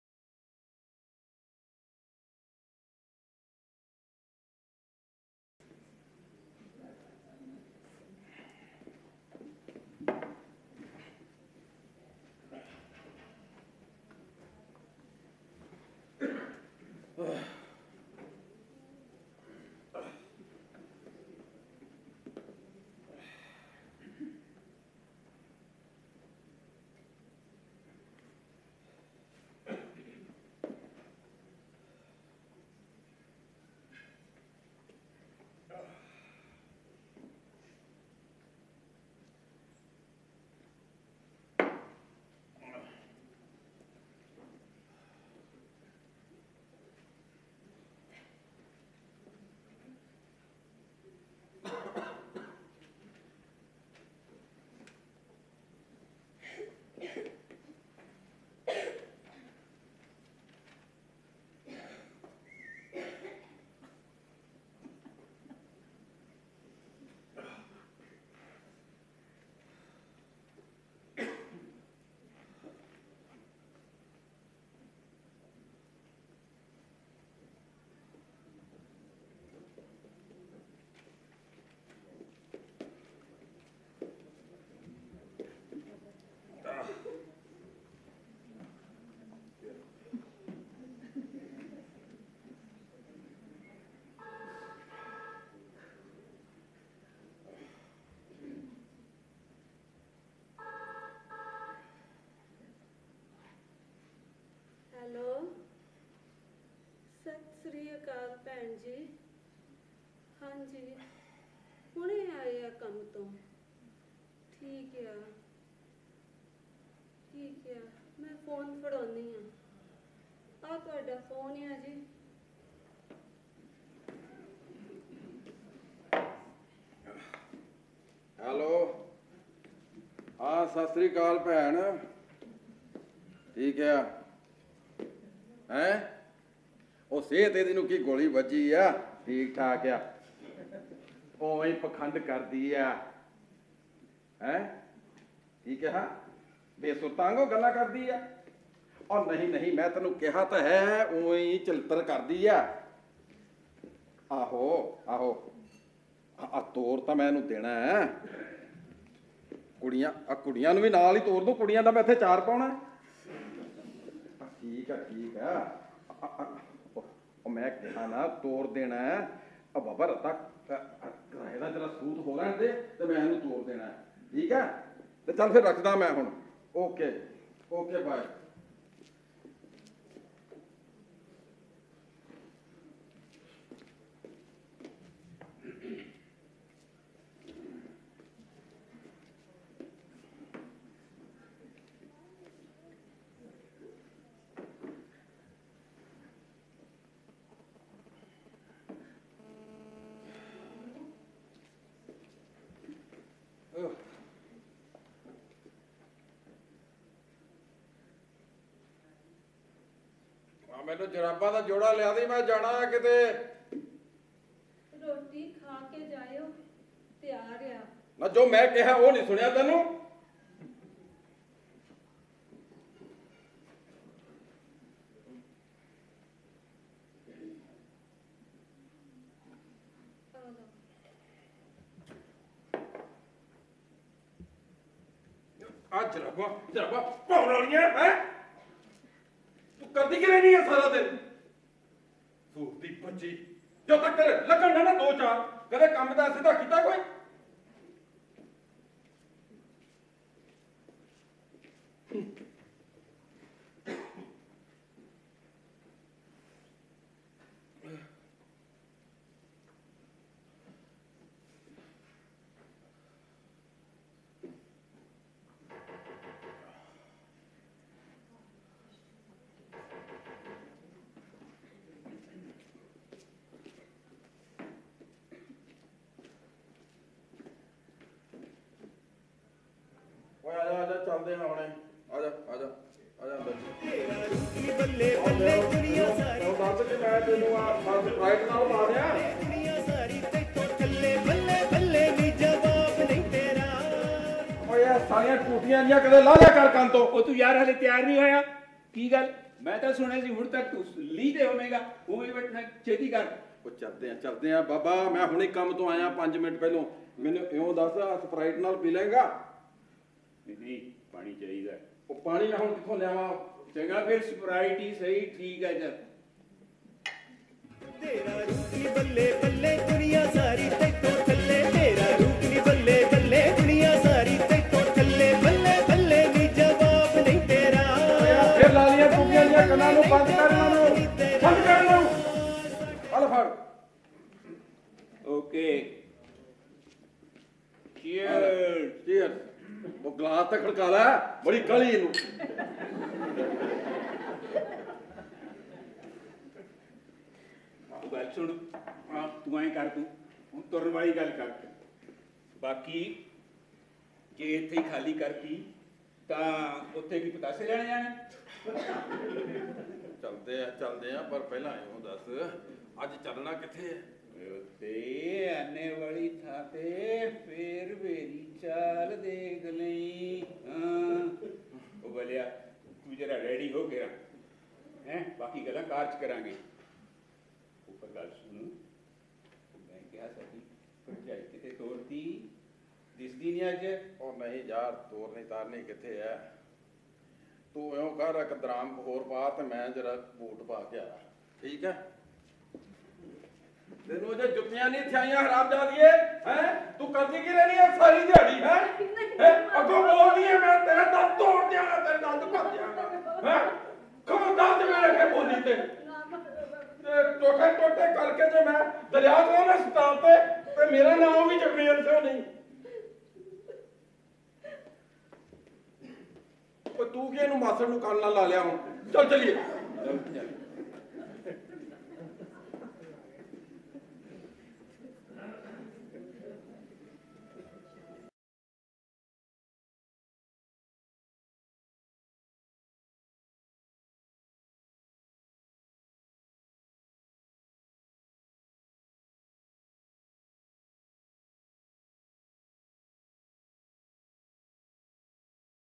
Ik Kurhi Ik Supna (One Girl One Dream) - Part 2
Ik Kurhi Ik Supna (one Girl One Dream) - a Punjabi Play directed by Dalvinder Multani